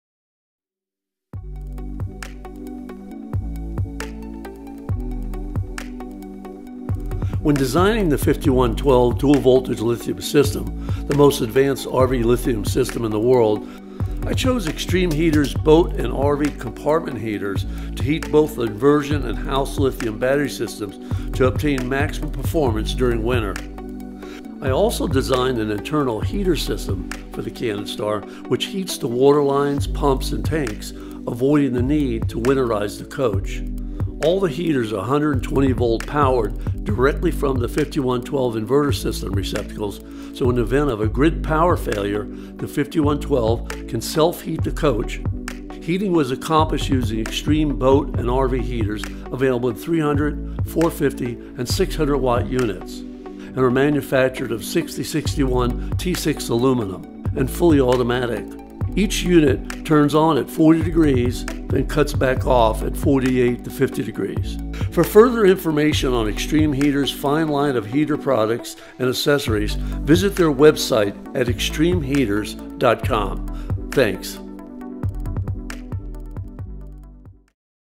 Xtreme RV & Marine Heaters
This video shows Xtreme RV Heaters. These RV & Marine heaters are Coast Guard Approved and the safest heater for RV protection in Winter. Xtreme Heaters are all metal construction and fully automatic, just plug in and forget. A built in thermostat turns the unit on and off automatically to protect your Lithium Batteries or RV in winter.